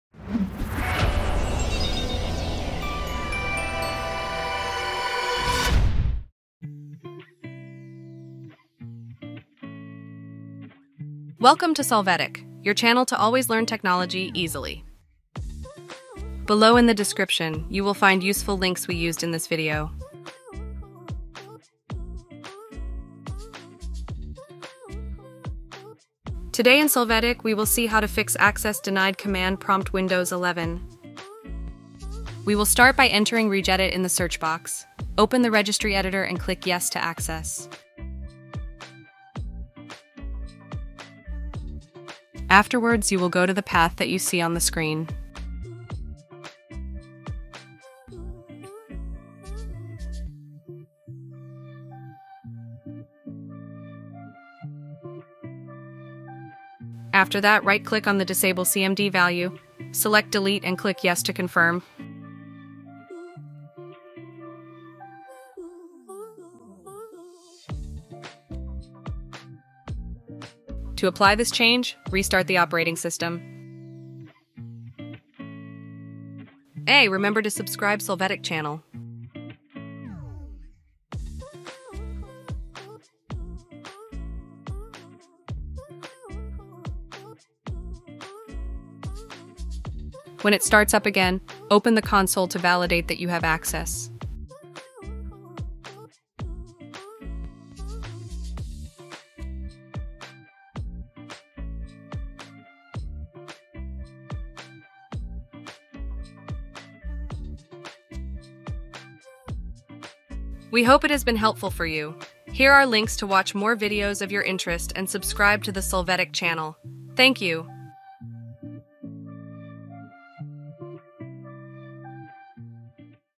Access Denied Command Prompt Windows 11
Hello everyone, and welcome to Solvetic! In this video, we'll show you how to fix the "Access Denied" issue in the Command Prompt (CMD) on Windows 11. The "Access Denied" message may appear when trying to execute certain commands in CMD due to permission restrictions or configuration issues. But don't worry, we're here to help you solve this problem quickly and easily. You'll learn different methods to troubleshoot the "Access Denied" error in CMD, including changing file permissions, running CMD as an administrator, and more. Don't miss out on this helpful tutorial!